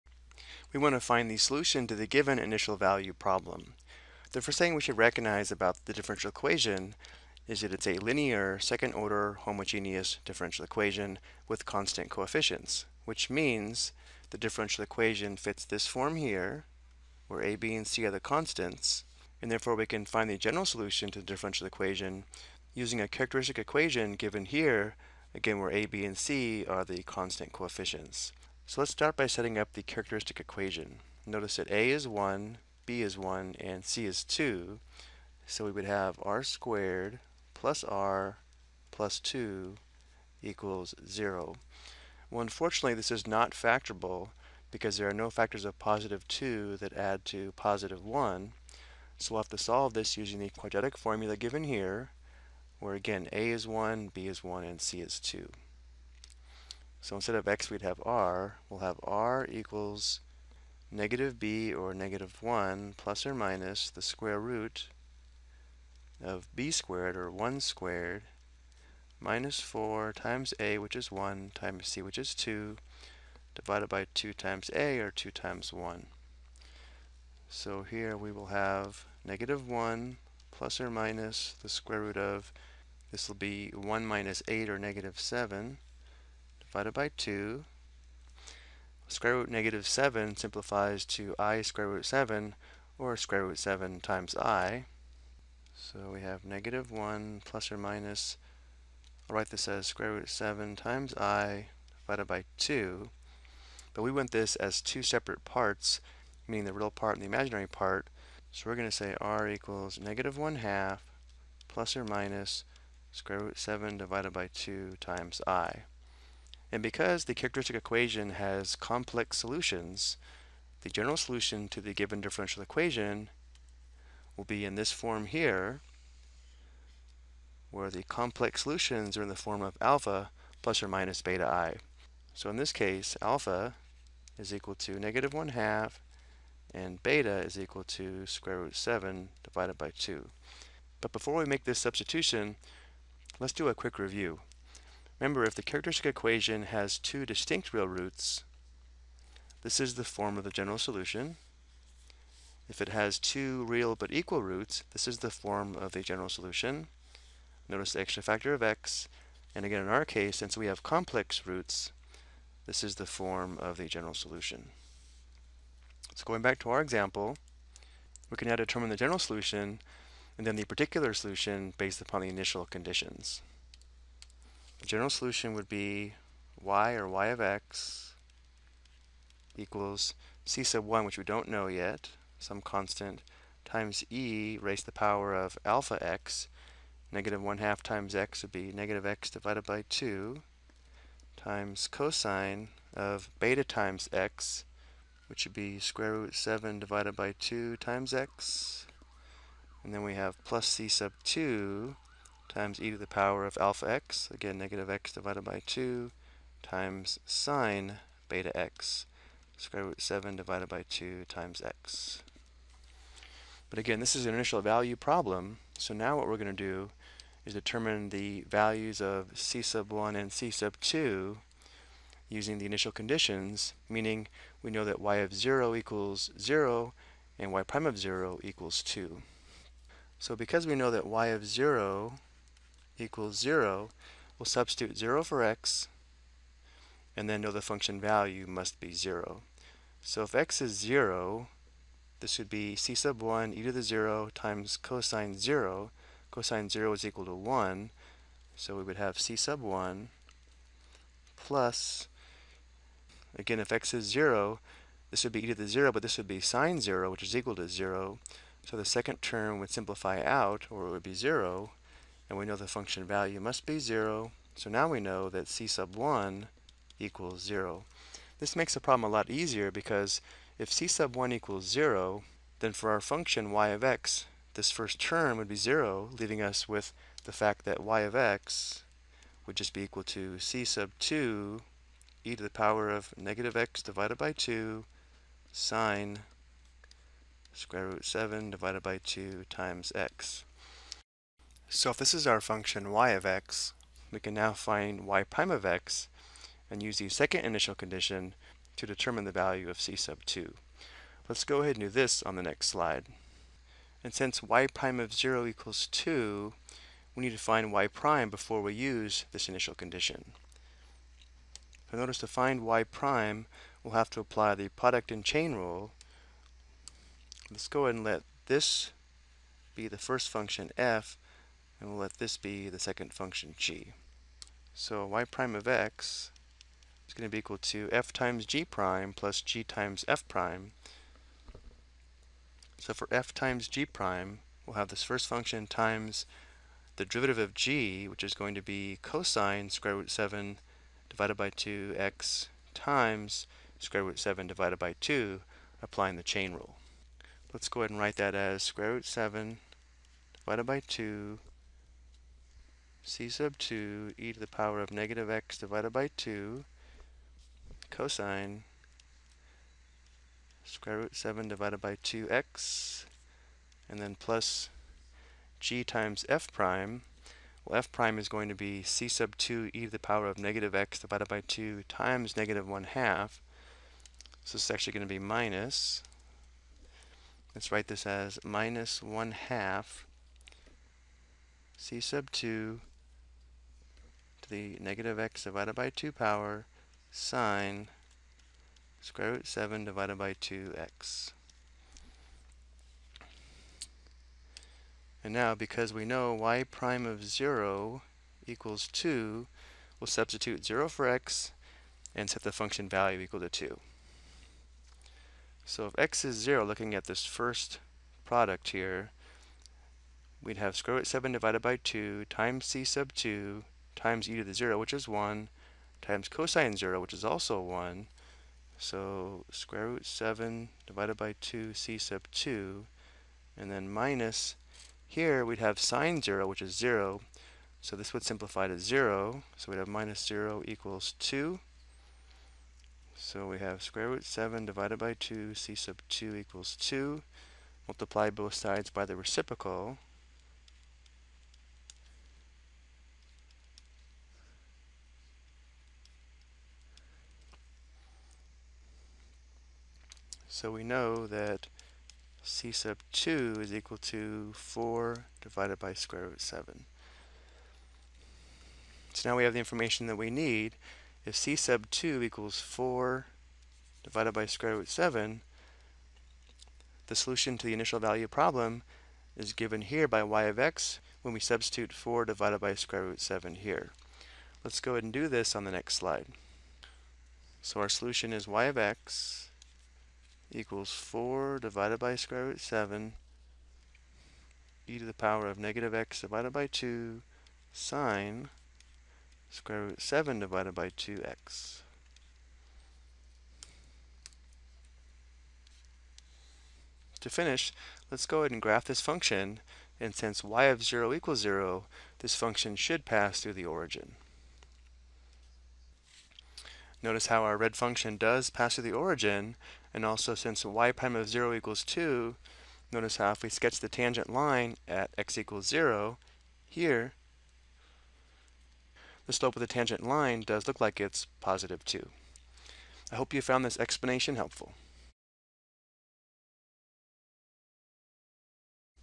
Ex: Solve a Linear Second Order Homogeneous Differential Equation Initial Value Problem (complex)
This video provides example on how to solve a linear second order homogeneous differential equation with constant coefficients initial value problem when the characteristic equations has two complex roots.